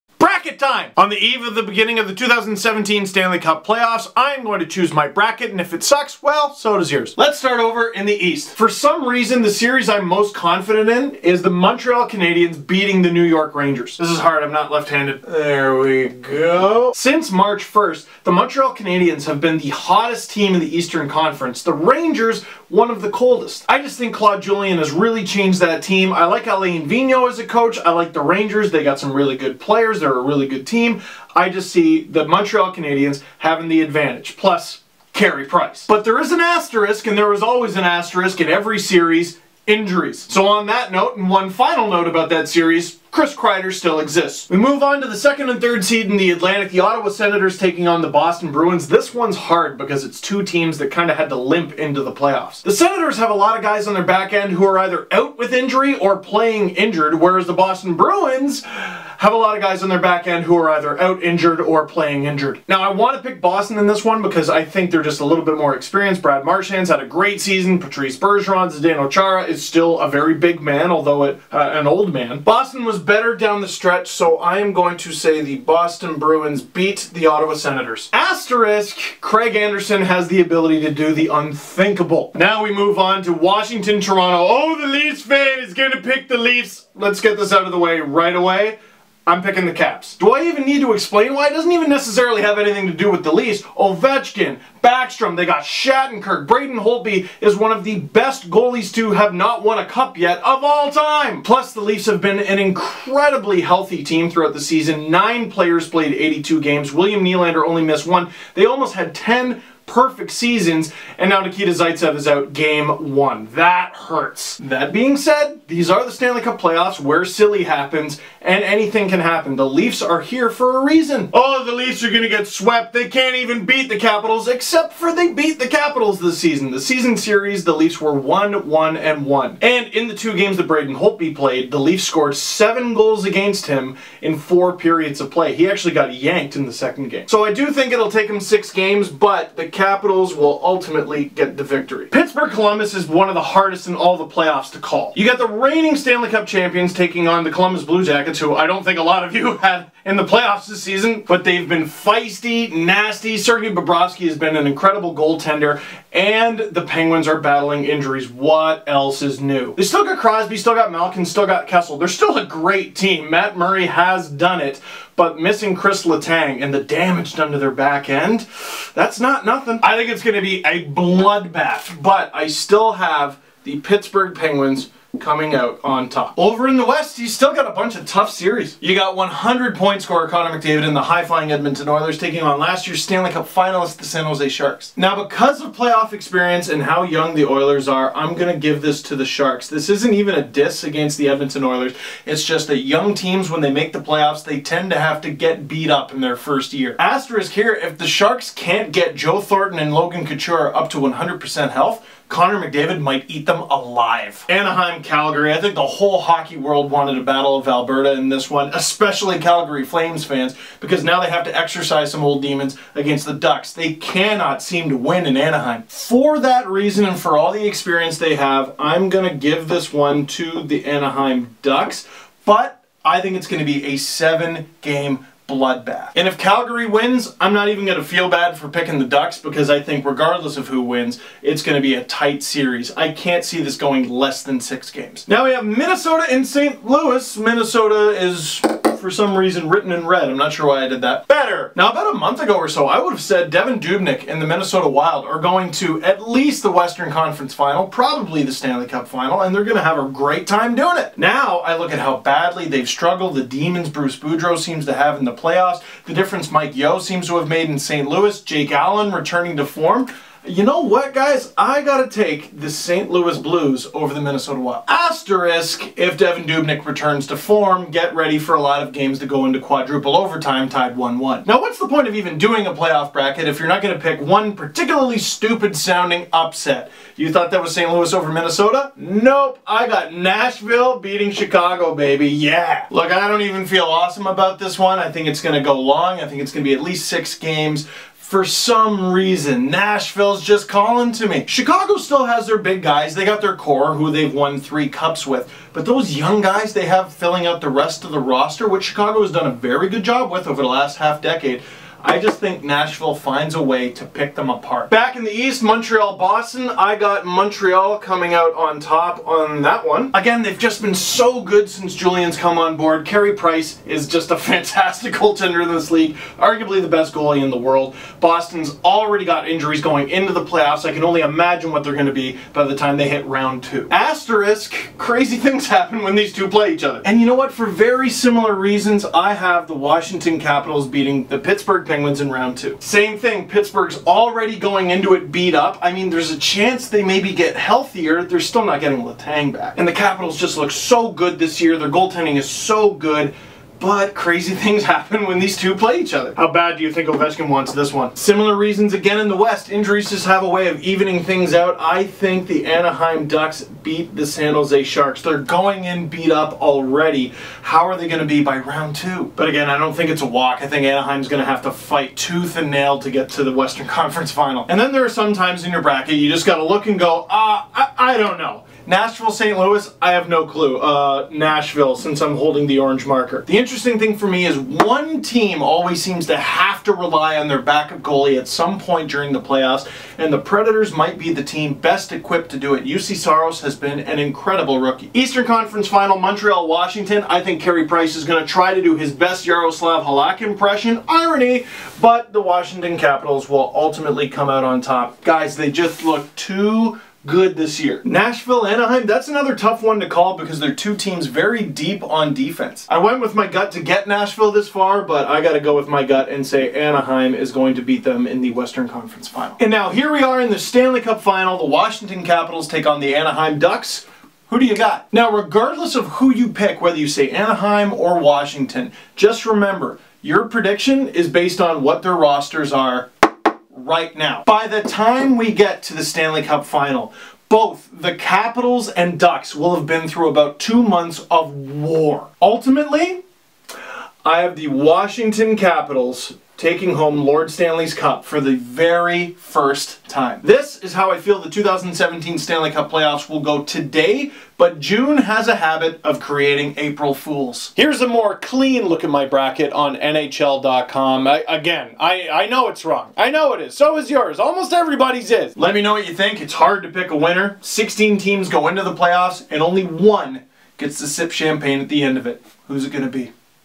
Stanley Cup Bracket Challenge with Steve Dangle
On the eve of the 2017 Stanley Cup Playoffs, Steve Dangle takes us through his bracket and how he thinks the playoffs will play out.